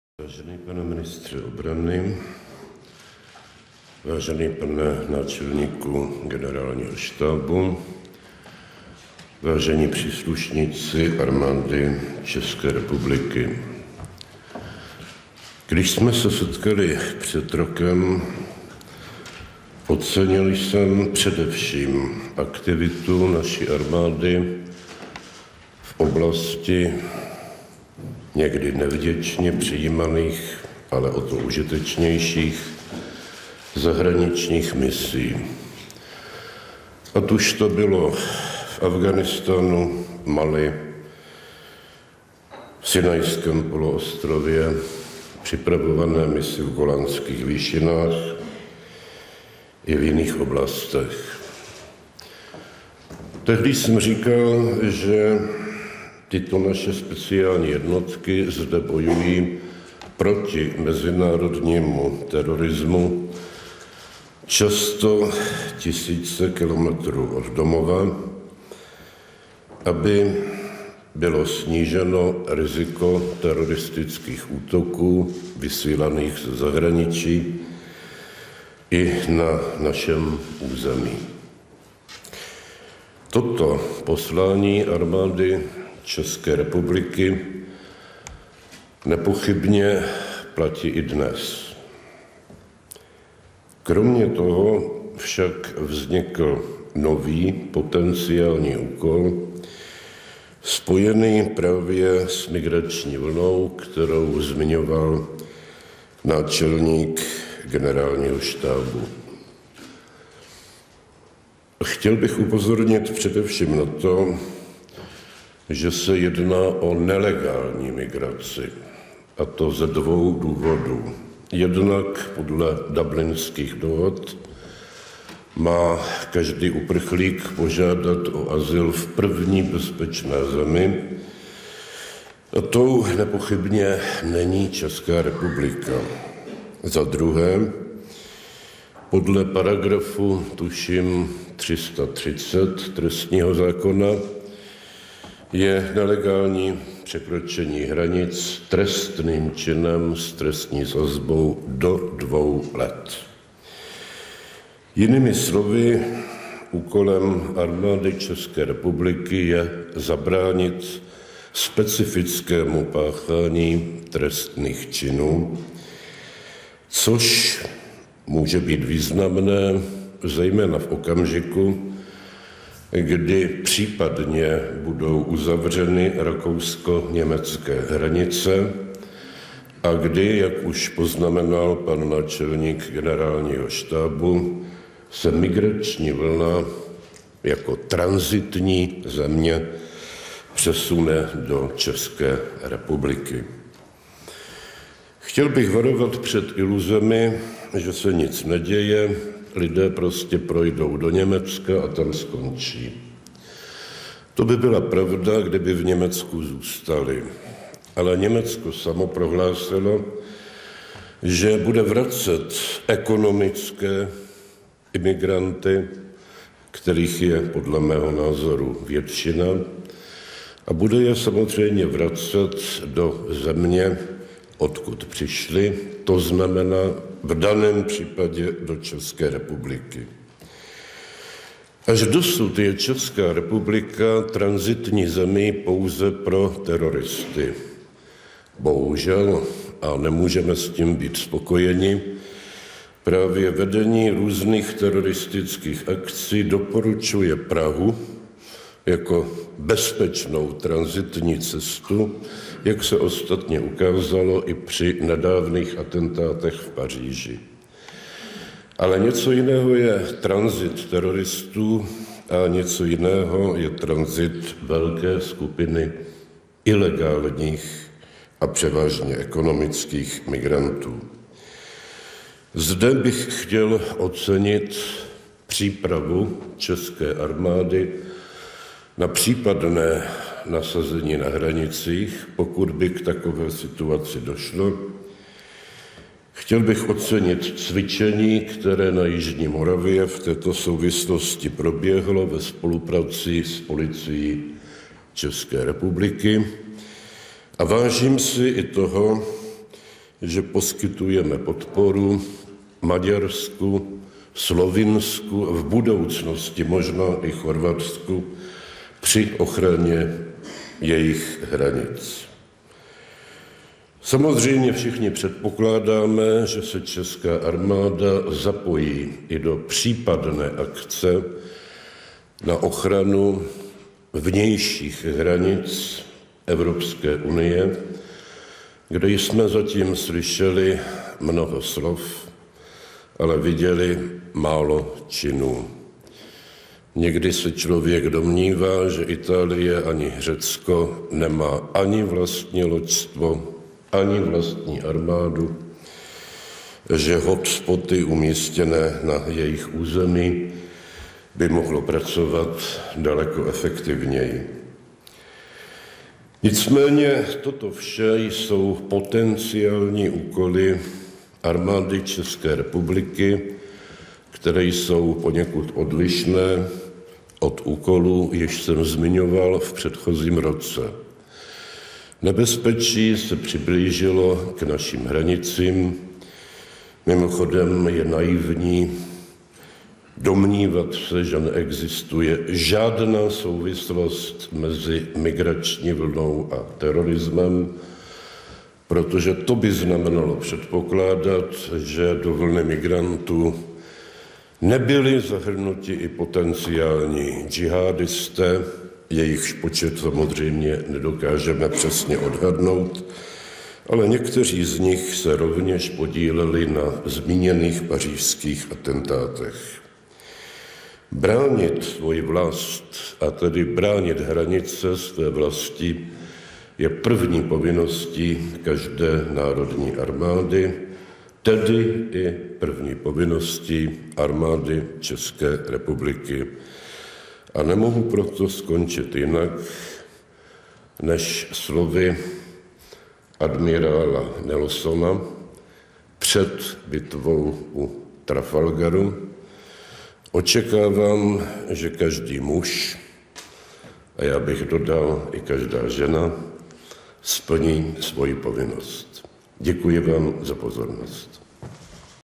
Zeman: Nebezpečí se přiblížilo k našim hranicím
Prezident ČR Miloš Zeman na velitelském shromáždění Armády České republiky.